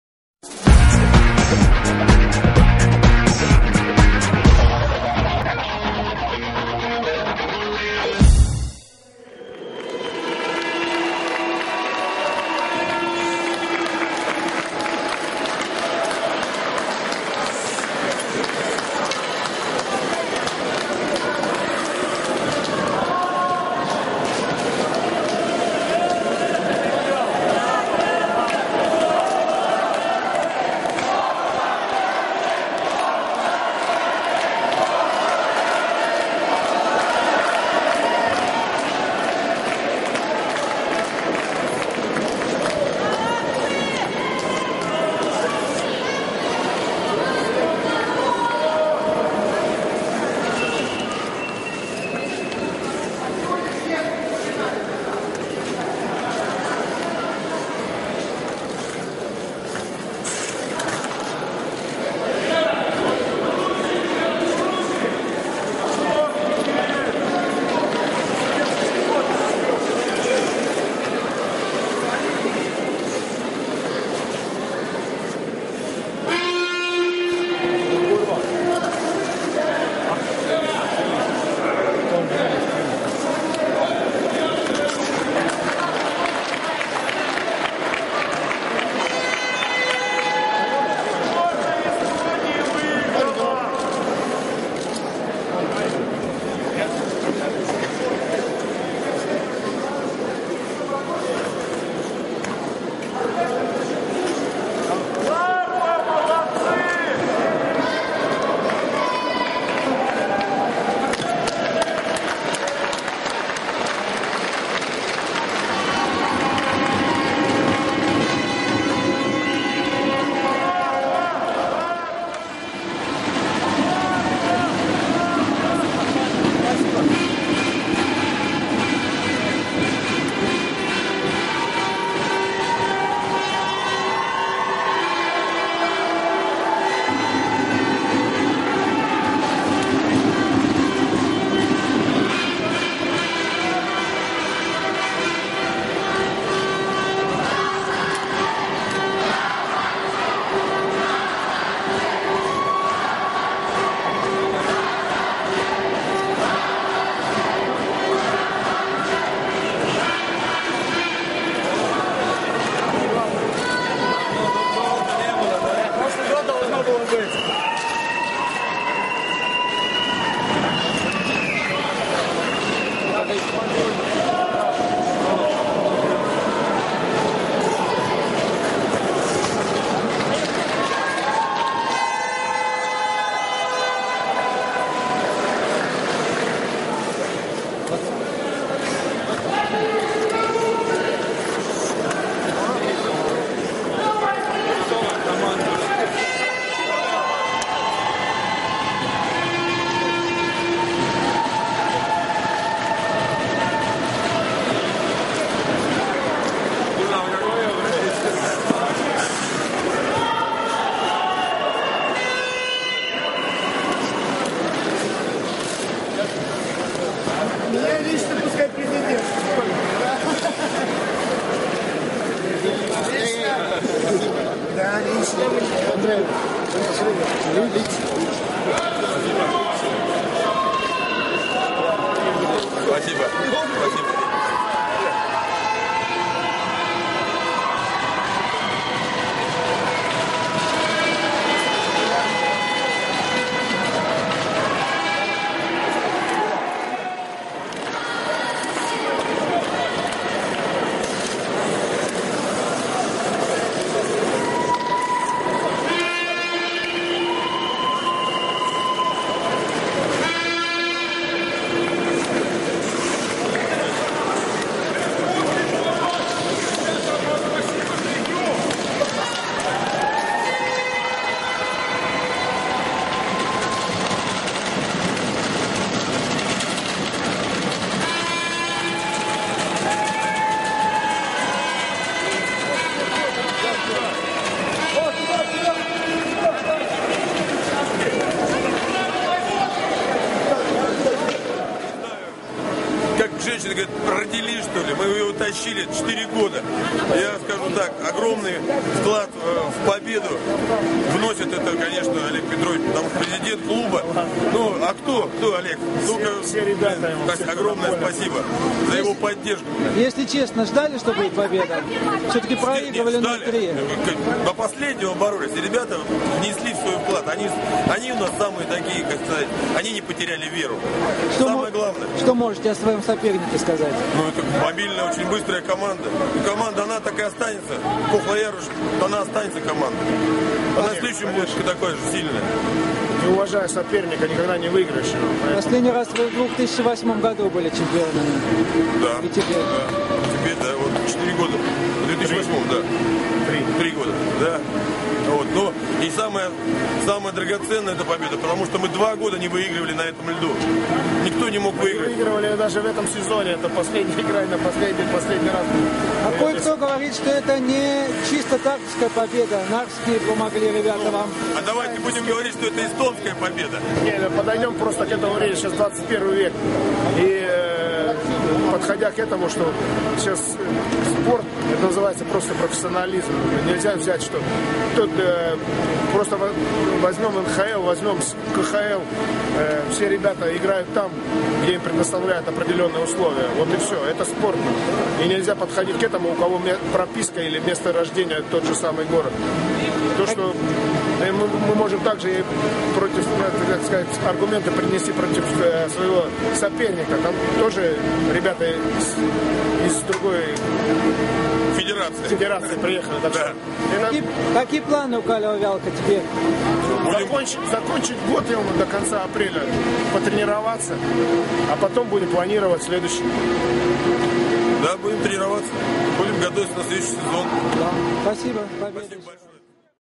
Финал чемпионата Эстонии по хоккею. 5 игра. 5 часть.
Jäähoki: Eesti meistrivõistluste finaali 5. mäng, Kohtla-Järve Viru Sputnik - Tartu Kalev/Välk. Toetus: Euroopa Kolmandate Riikide Kodanike Integreerimise Fond, Integratsiooni ja Migratsiooni Sihtasutus Meie Inimesed ja Kultuuriministeerium